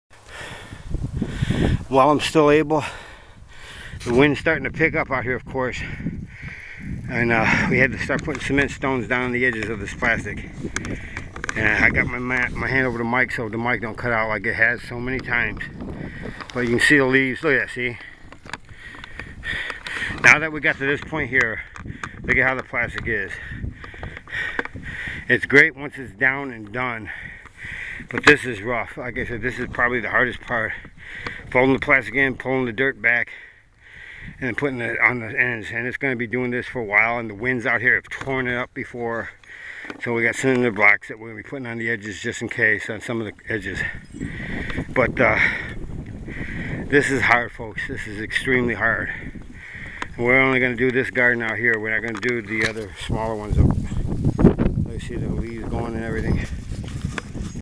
58 seconds Very short semi-finish GARDEN PREP
This minute or so video shows close to how it was finished. After tilling then putting the plastic sheet down on our LARGEST #3 garden, we had TILLED ALL 3 and put down plastic on the EAST/#2 GARDEN, AFTER THIS ONE.WE stopped and waited till next day BUT IT RAINED  and we have yet to do the WESTERN/#1 GARDEN  yet, WE'LL HAVE TO TILL the edges again "WHEN IT DRIES! fold edges in and pull soil to outer area to THEN BURY the  edges "of"-  the plastic sheet. 5-6 YEARS BACK, it was rough... NOW... IT IS  VERY  ROUGH! ! !